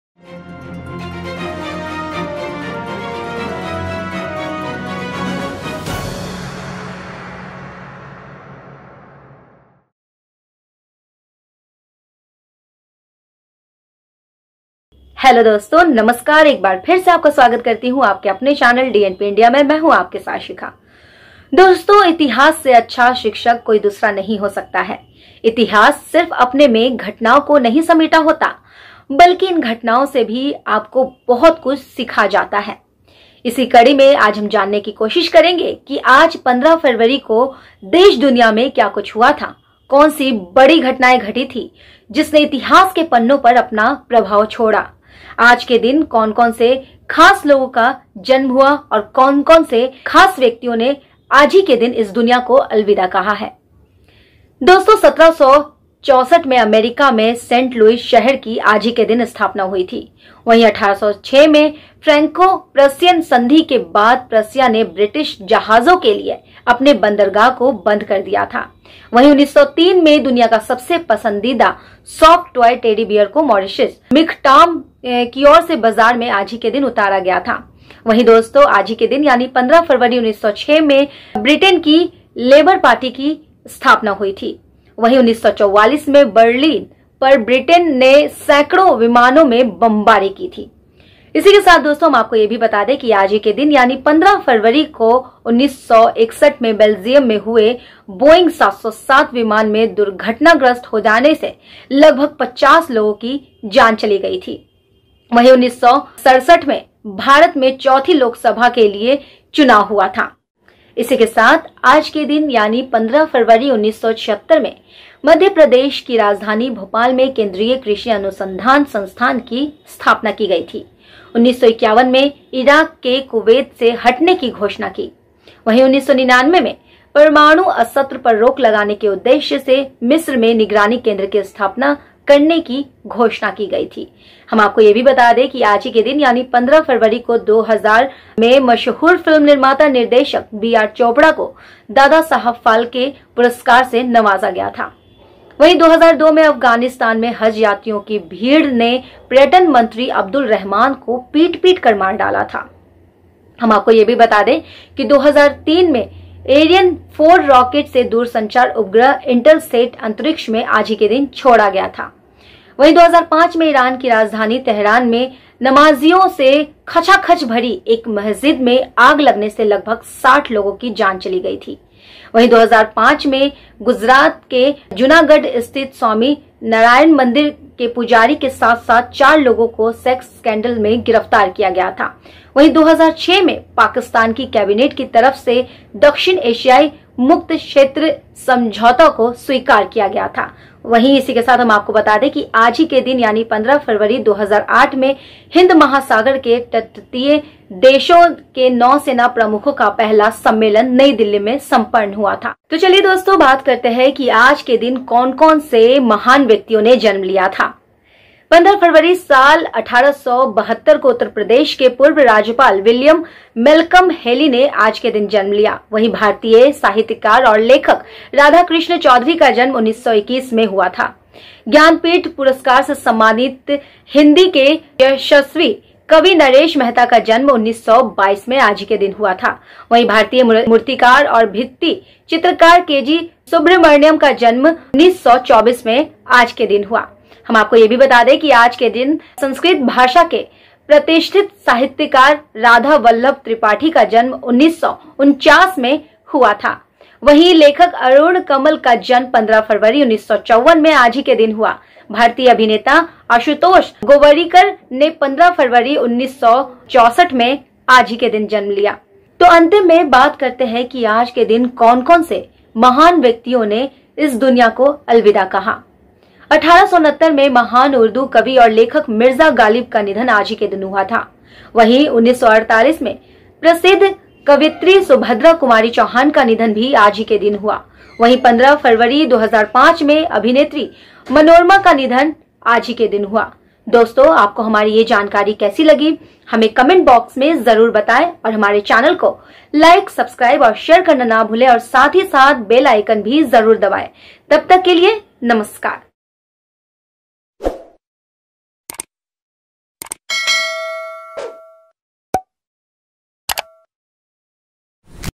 15 फरवरी को देश दुनिया में क्या हुआ ?
मिर्ज़ा ग़ालिब की शायरी से मनोरमा की कॉमेडी तक